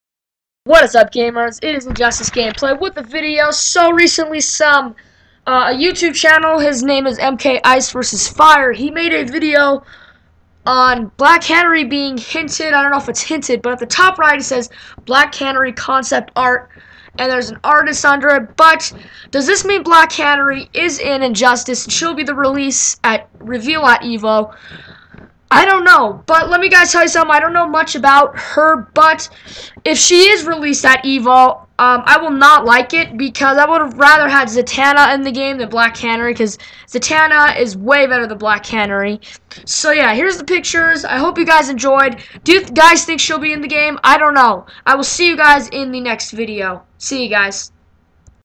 Injustice: Gods Among Us Black Canary Revealed?
Is black canary the reveal at EVO? I wanted Zatana but oh well. At least its a better pick than Power girl or Sub Zero...